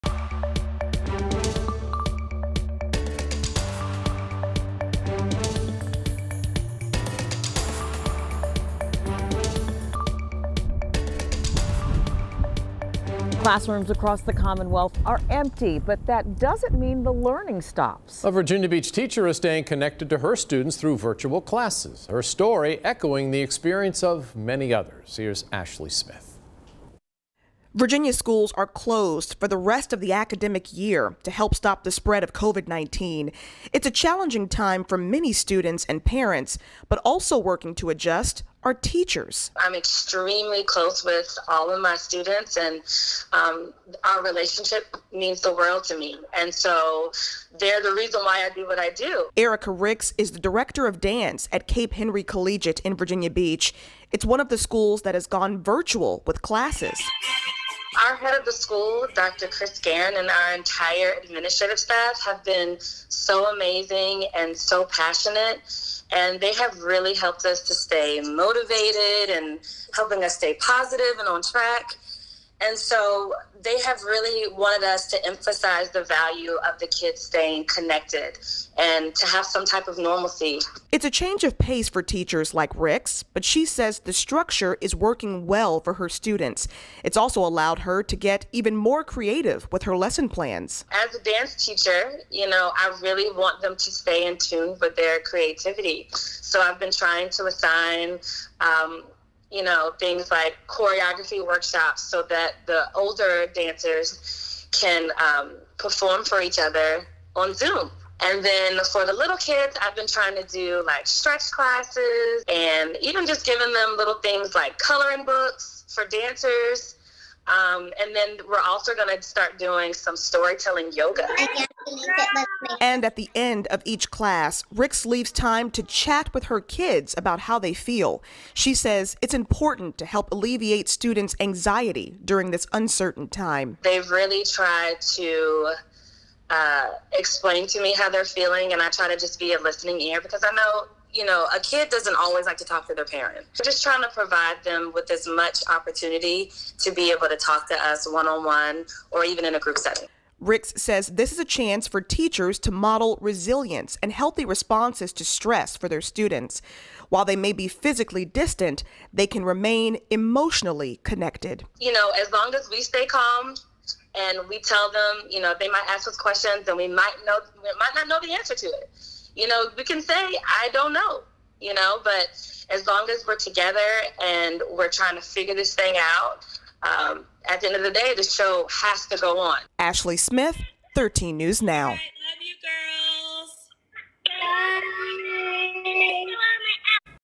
Virginia Beach teacher connects with students virtually amid COVID-19 outbreak
Virginia has closed schools for the rest of the year due to coronavirus concerns. But for Ericka Ricks’ classes, that doesn’t mean education stops.